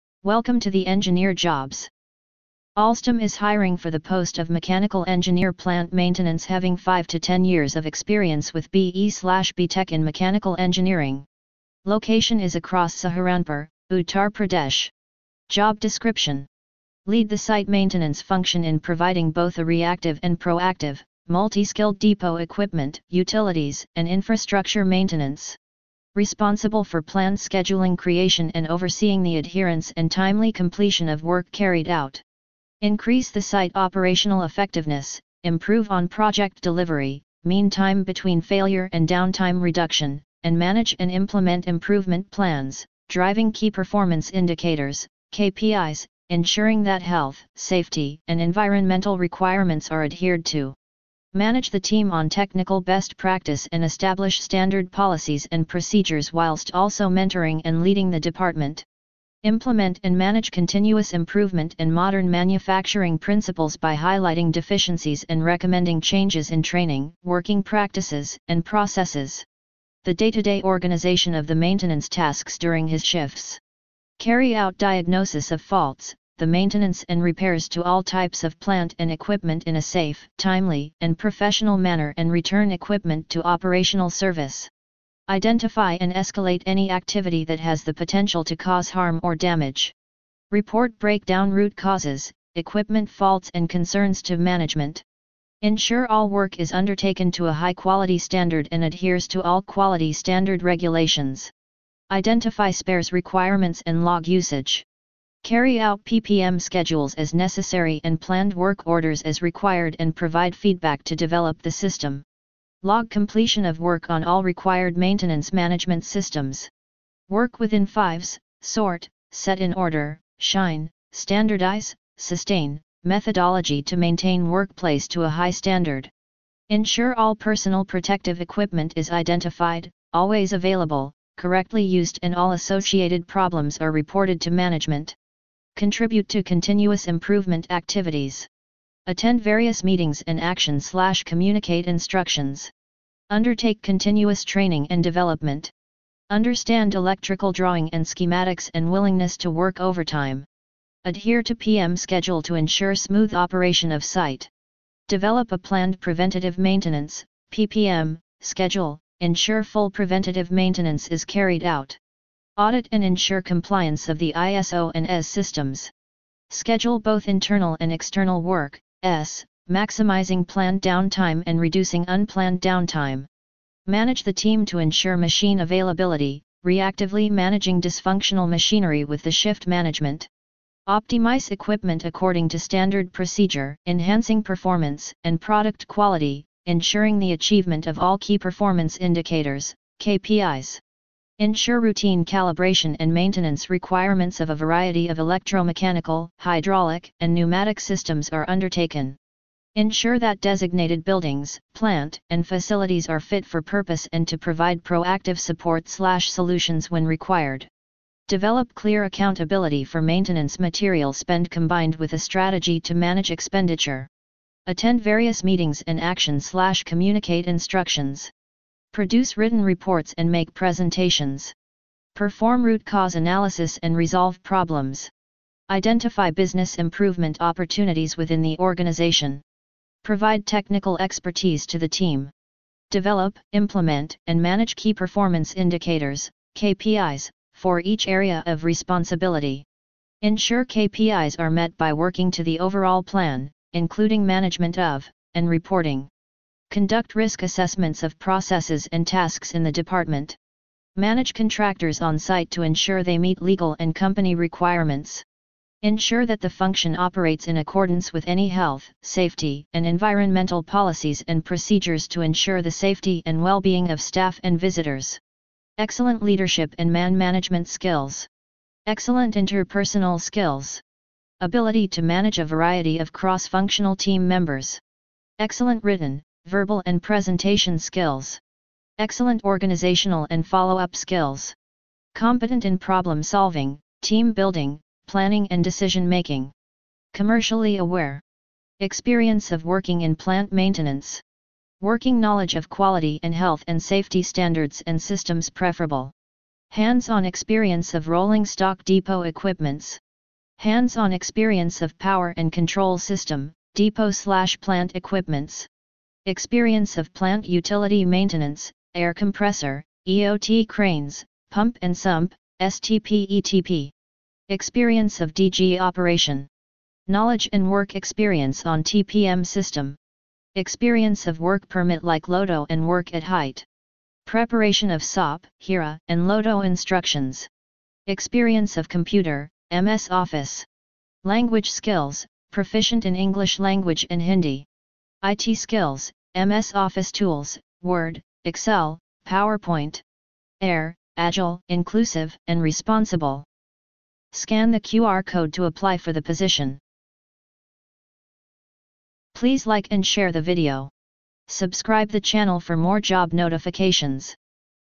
ALSTOM | Mechanical Engineer Plant Maintenance | BE/BTech - Mechanical | Saharanpur, Uttar Pradesh
Scan the QR Code in the video to Apply for the position.